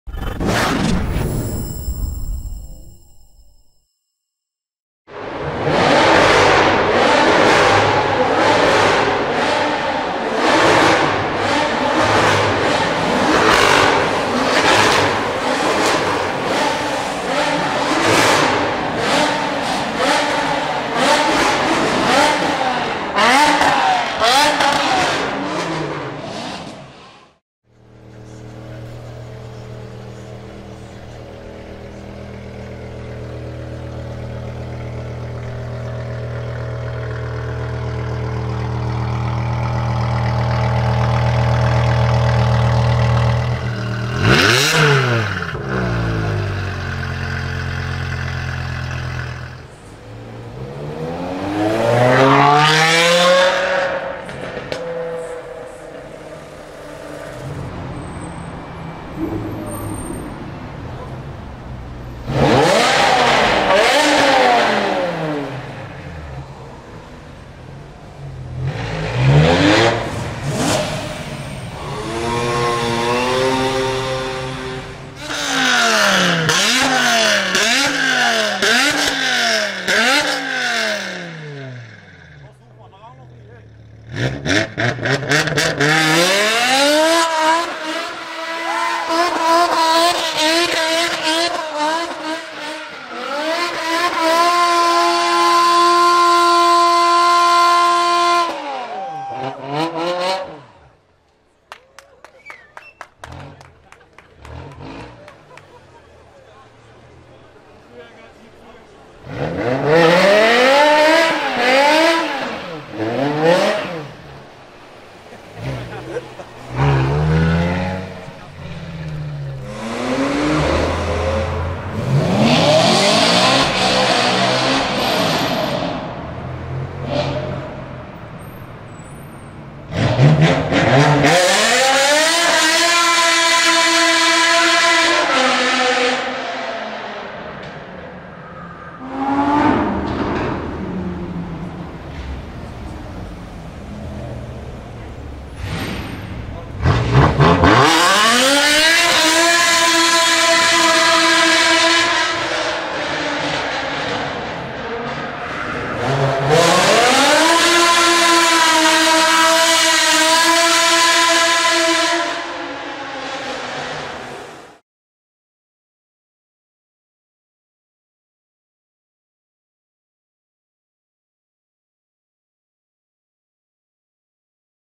BMW M5 E60 with Eisenmann Race Exhaust INSANE Launches & Burnouts!!
During the Monaco Top Marques 2017 I have filmed this BMW M5 E60 fitted with an insanely LOUD Eisenmann Race Exhaust! Video shows the car doing some badass launches, burnouts, revs and accelerations in a tunnel!! Make sure you turn up the volume and listen to the orgasmic V10 sound!!